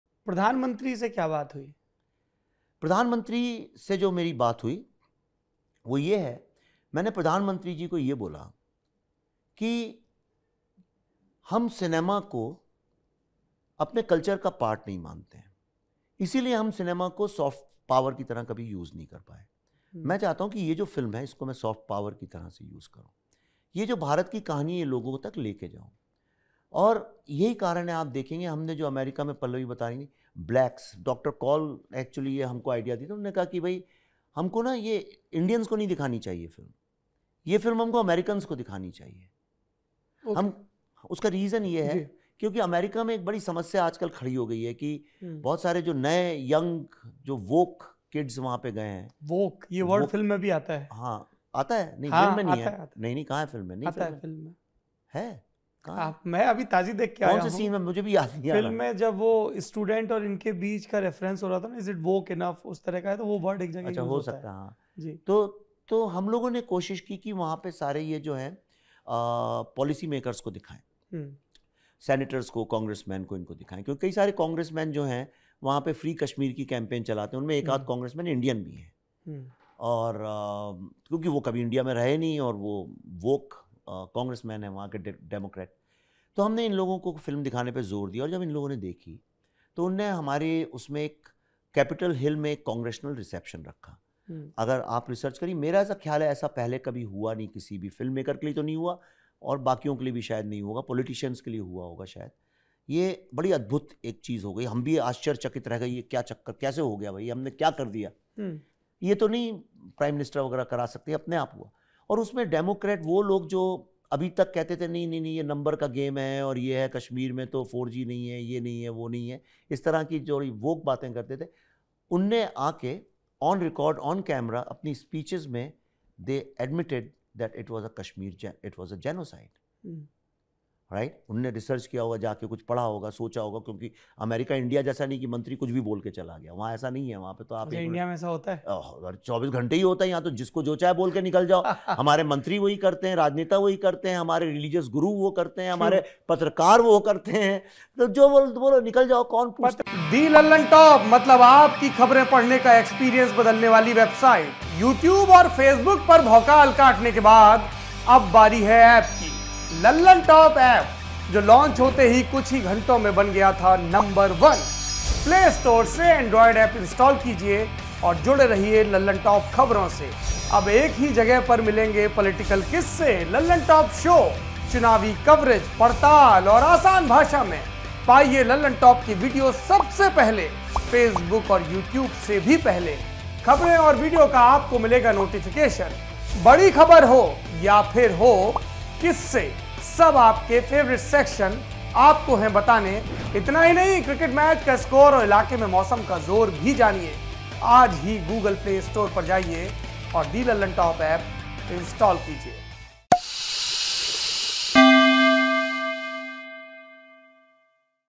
जब The Kashmir Files के डायरेक्टर PM Narendra Modi से मिले । Vivek Agnihotri । Movies soft power
Saurabh Dwivedi interviews The Kashmir Files director Vivek Agnihotri. In this snippet of conversation Vivek talks about his meeting with PM Narendra Modi after the release of his film. He spoke to PM Modi about using The Kashmir Files as soft power. Before releasing his film in India, Vivek Agnihotri showed it to American bureaucrats. After watching The Kashmir Files they organised a congressional reception in capitol hill for the filmmaker. In this event they admitted on record that what happened in 1990 in Kashmir was genocide. Watch this video to know more.

Edited by: